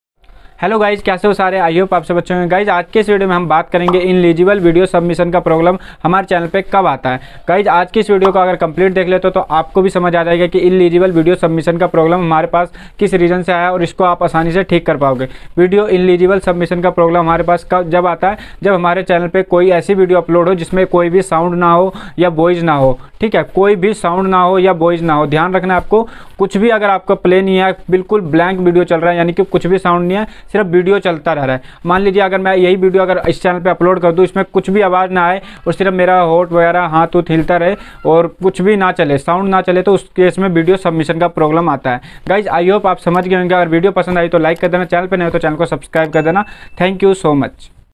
How to ineligible video submission || ineligible video submission problem
Style tech
styletech How to ineligible video submission || ineligible video submission problem

doston is video mein maine ineligible video submission ka problem kaise aata hai iske bare mein bataya hai video pasand aaye to apni taraf se like channel ko subscribe jarur Karen thank you

About Video Query 👇

How to ineligible video submission ||
ineligible video submission problem ||